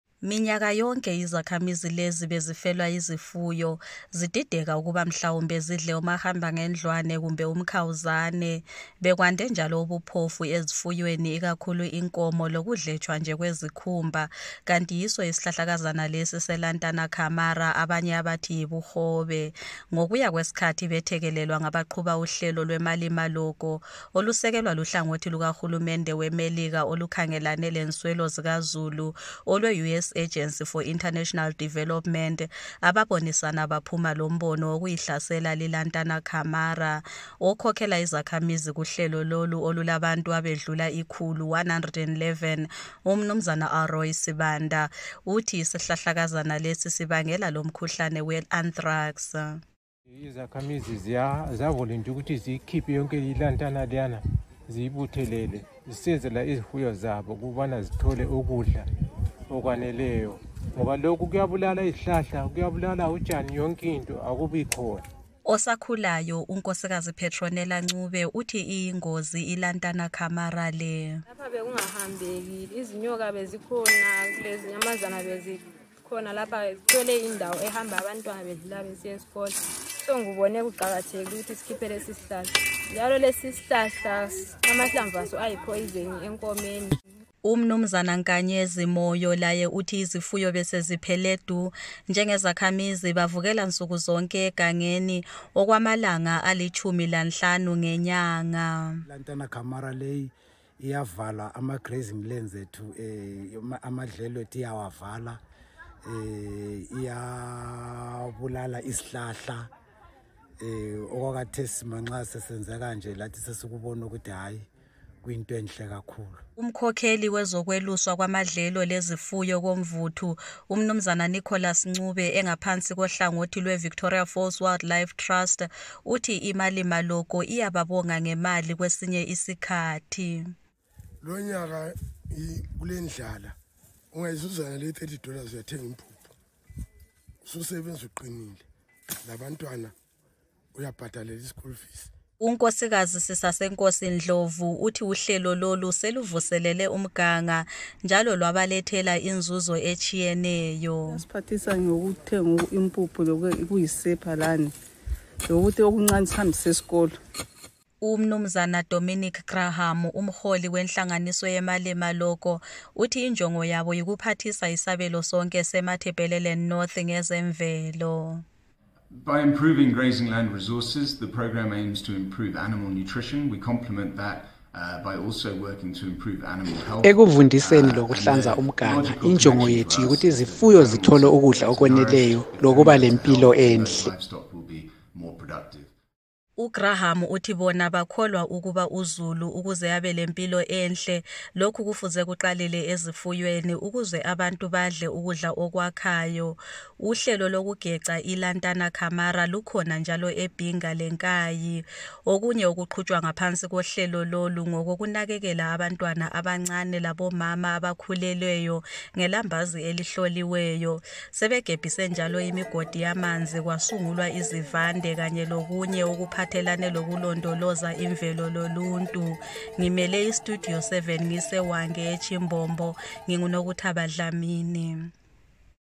Izakhamizi Zigamula iLantana Kamara Ebulala Izifuyo
Izakhamizi zeHwange ebezifelwa yizifuyo ziqhuba uhlelo lokugamula okuyisihlala okubizwa yikuthi yi-lantana camara, abanye abathi yibuhobe. Uhlelo lolu lusekelwa luhlangothi lweMelika olubona ngezentuthuko olweU.S Agency for International Development. (Video: Nokuthaba Dlamini/VOA)